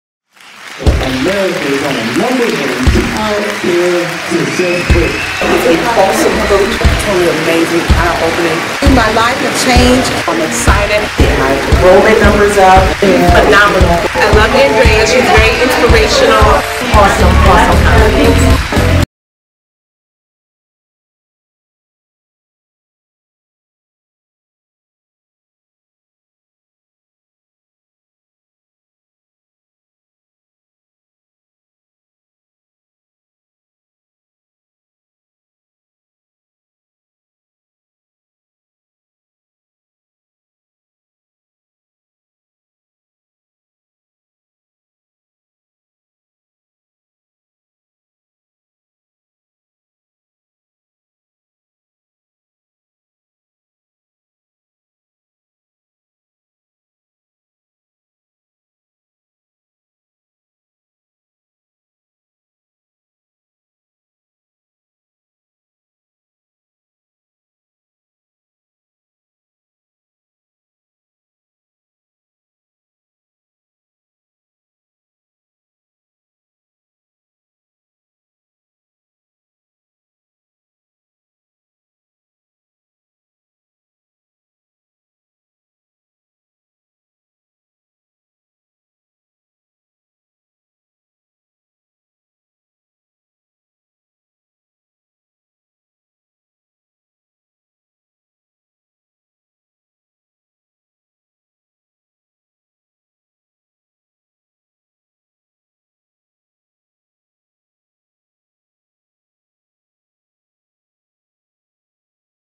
The Build It Big Conference For Childcare Owners!
Join Andrea For A 2 Day Jam Packed Conference To Inspire, Motivate and Provide You With Information To Succeed! New and Improved Conference!